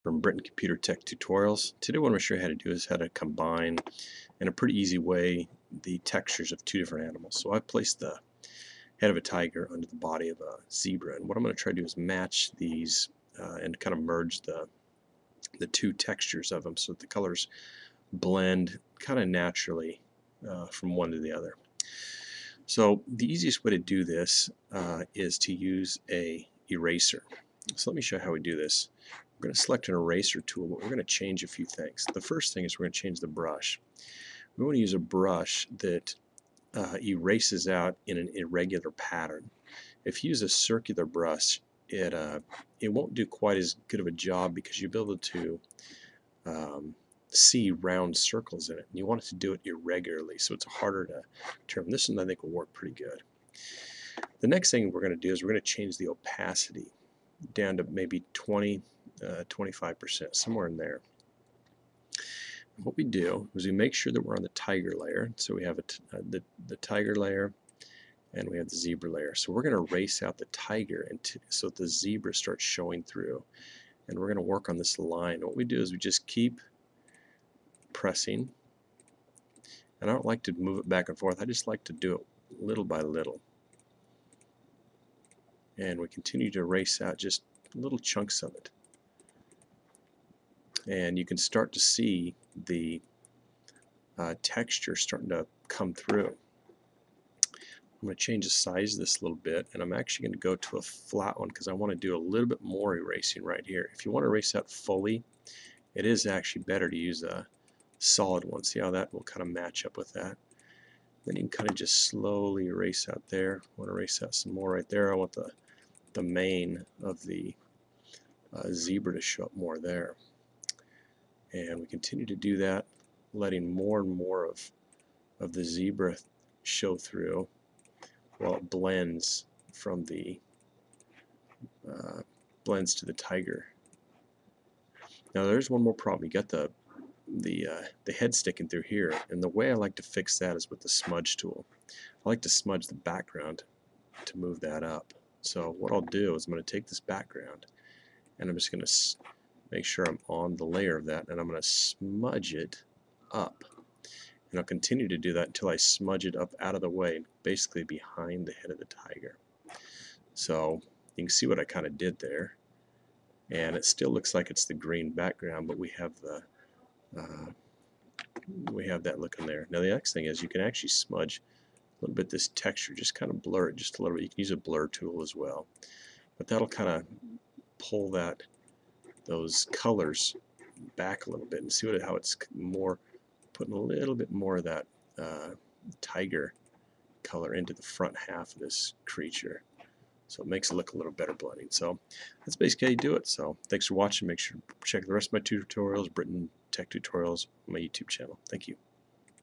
Photoshop CS5 how to combine the textures of a zebra and a tiger
In this tutorial I will show you how to use Photoshop CS5 to combine the textures of a zebra and a tiger.